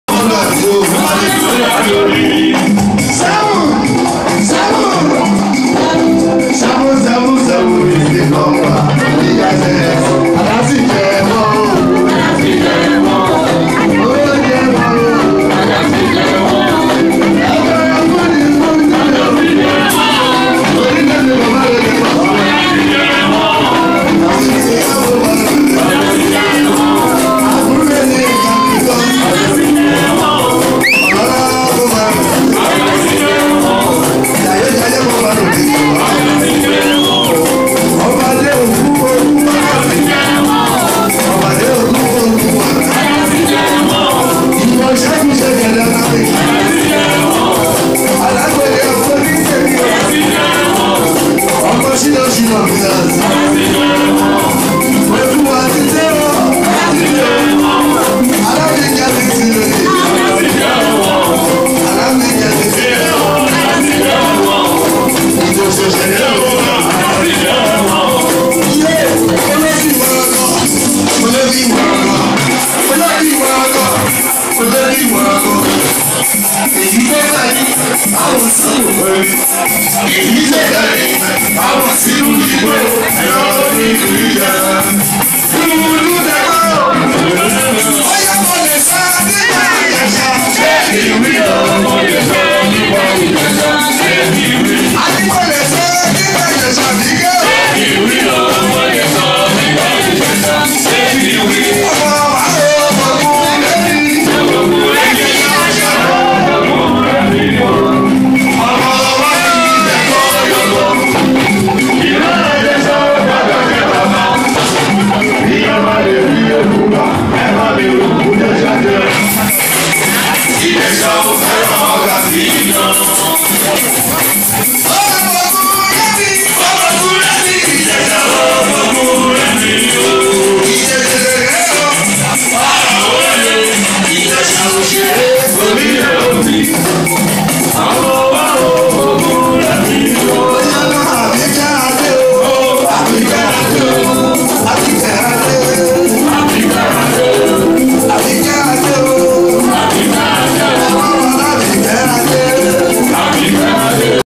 Sir Shina Peters London Launching of Odua Progressive Union
With BnS and Gani Adams